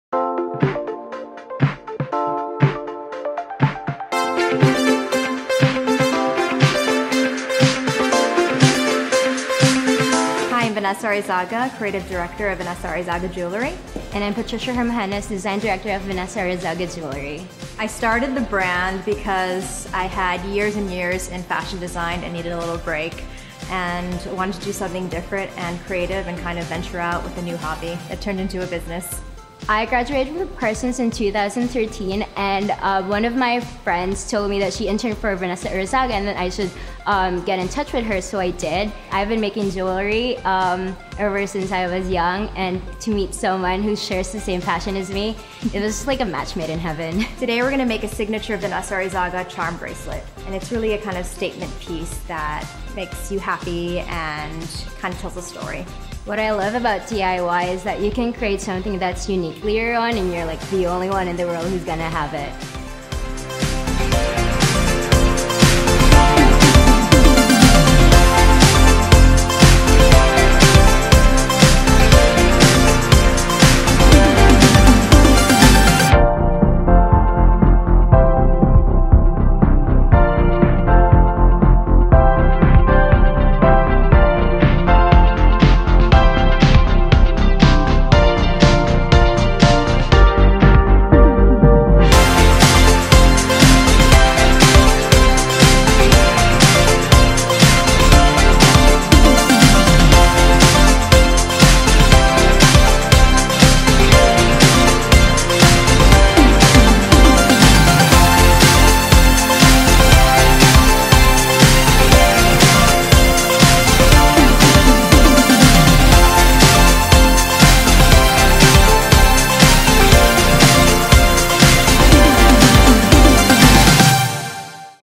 How to Make Your Own Charm Bracelet | Teen Vogue
Venessa Arizaga and Patricia Hermogenes, of Venessa Arizaga Studio, demonstrate how to make a charm bracelet at home.

Sound: Alberto Leon

Hair: Peter Matteliano using Oribe

Makeup: Colleen Runne using MAC Cosmetics

Nails: Yukie Miyakawa using Essie